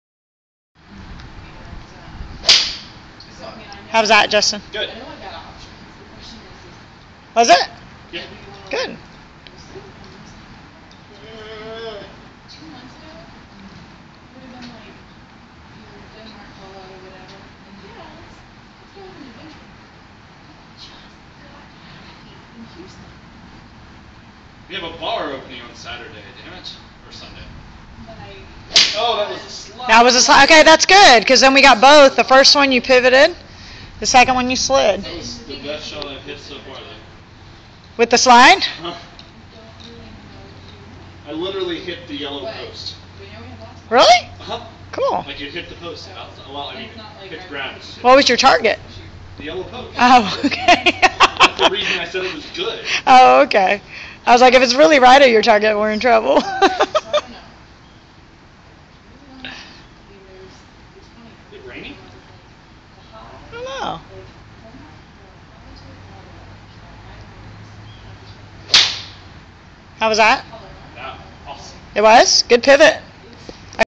Justin g(2/2012)
Tilt at hips with hands pushed down at setup; pivot/turn on back swing (vs slide to right); watch hand rotation at impact